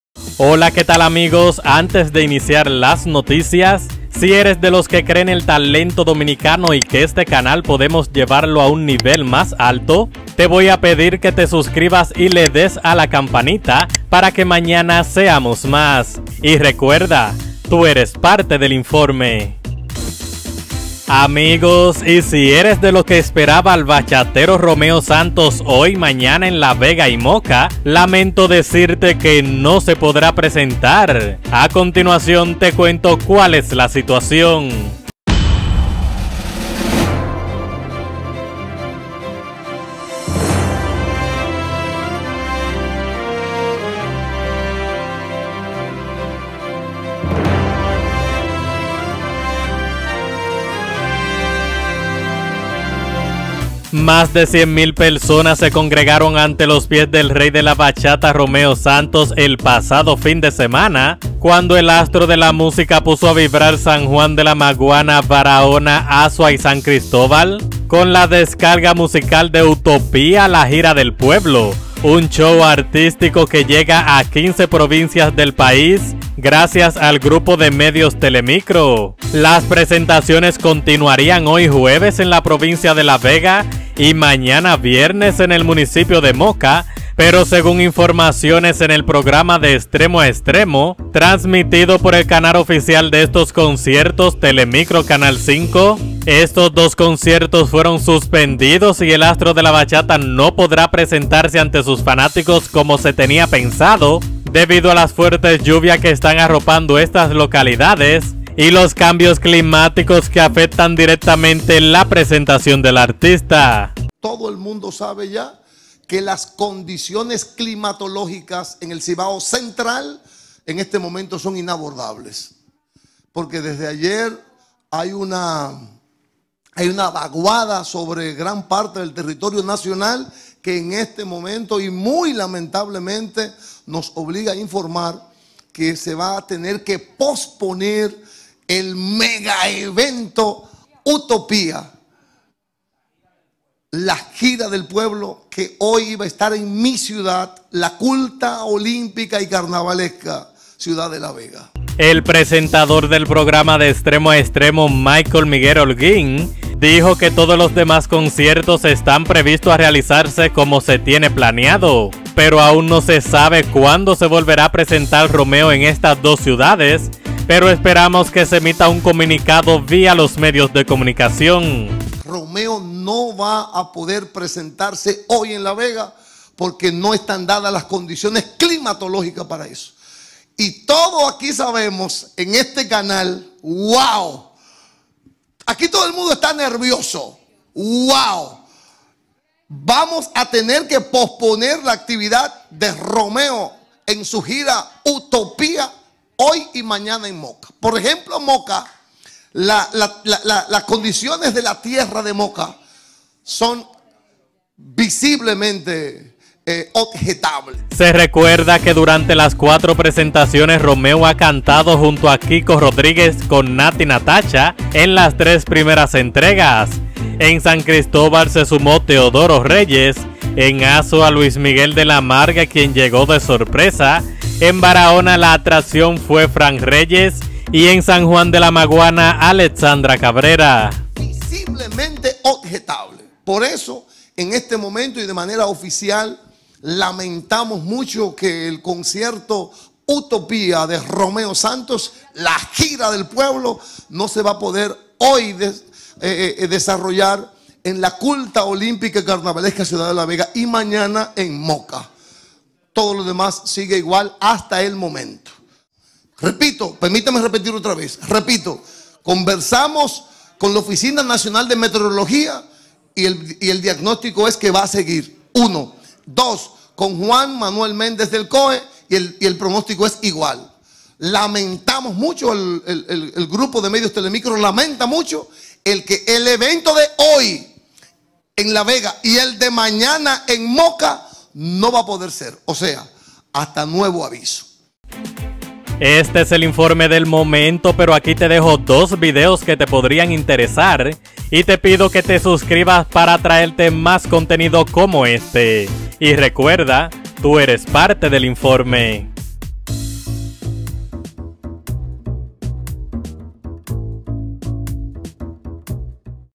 ROMEO SANTOS Cancela Dos Conciertos en el Pais, MIRA POR QUÉ.
El Bachatero Dominicano Romeo Santos, canceló dos presentaciones en el pais debido a los cambios climáticos.